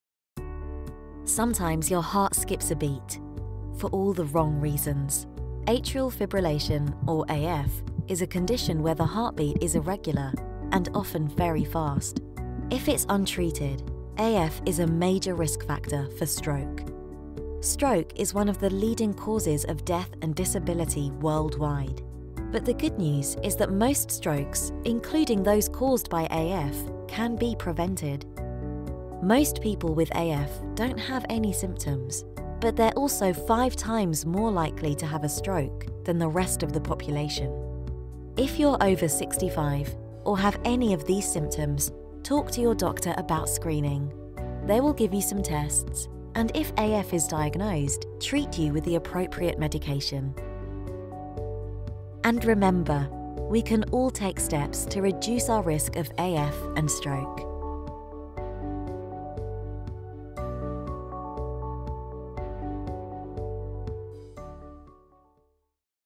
World Stroke Organization
People with Atrial Fibrillation are 5x more likely to have a stroke than people without the condition. Primary prevention, screening and medication reduce the risk associated with AF.